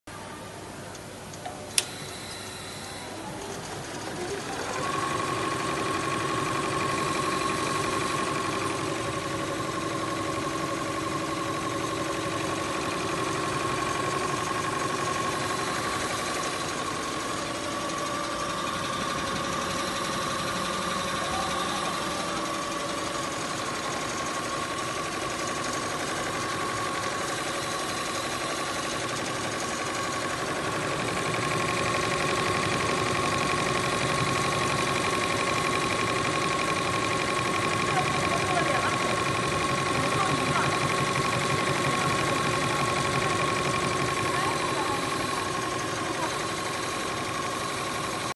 Richpeace embroidery machine do embroider works on jeans fabric 정밀 자수기
Richpeace embroidery machine do embroider works on jeans fabric
정밀 자수기
상표, 패션, 커튼, 침대보, 장난감, 장식, 자동차 인테리어 자수, 로고, 예술 및 공예품의 정밀 자수 공정에 적용됩니다.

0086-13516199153